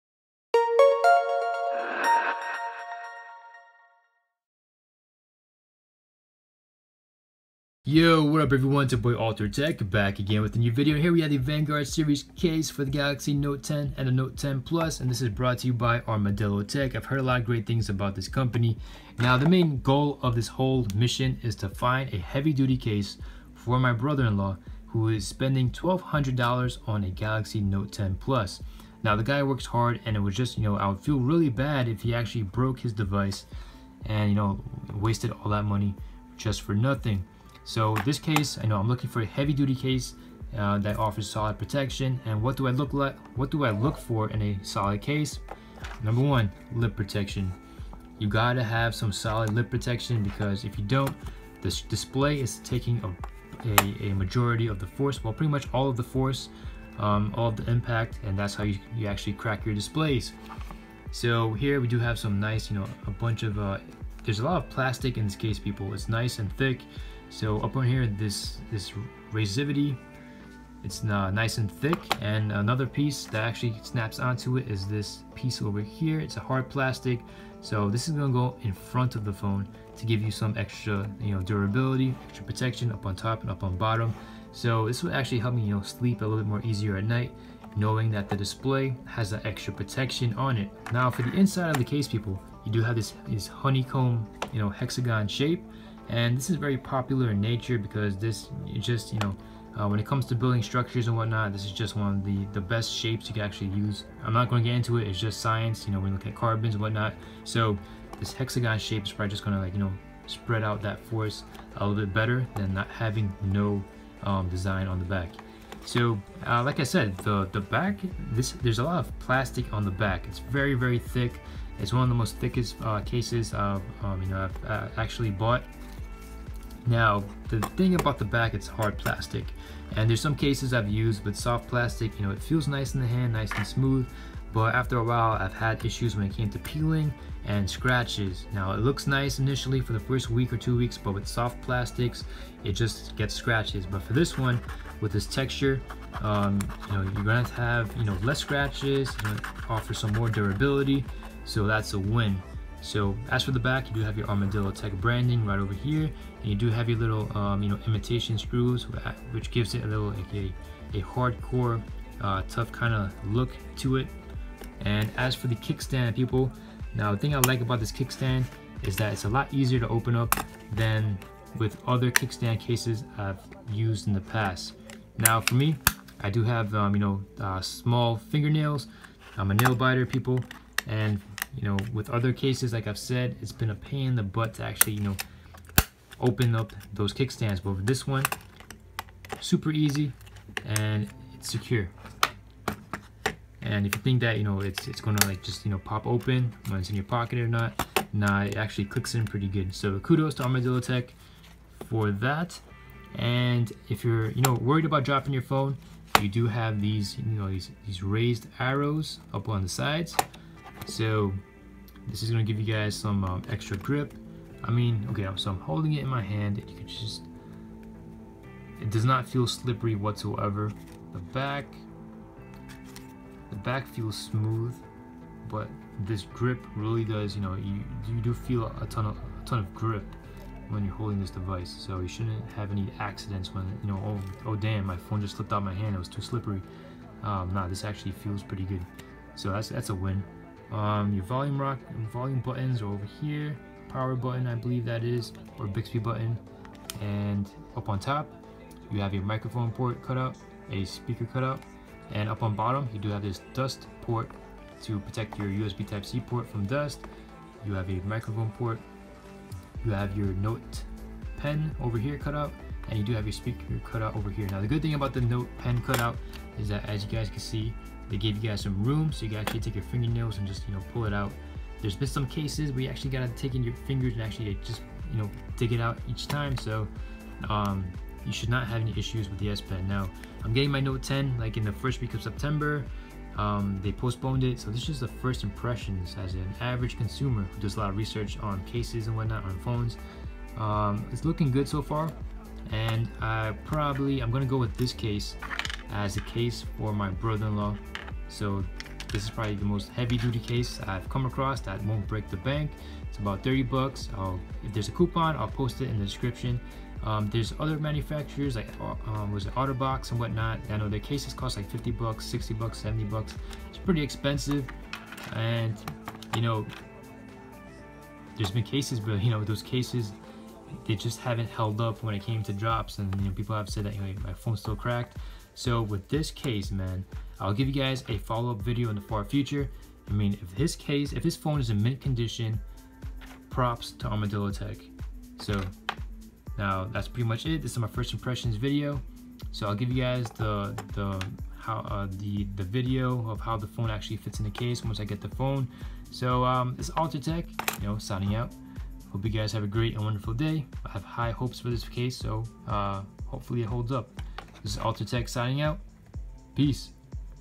Vanguard ArmadilloTek Case for the Note 10 and 10 Plus First Impressions!
Here are the reasons why I went with the Vanguard ArmadilloTek case for the Galaxy Note 10 and 10 Plus and what I think about it so far.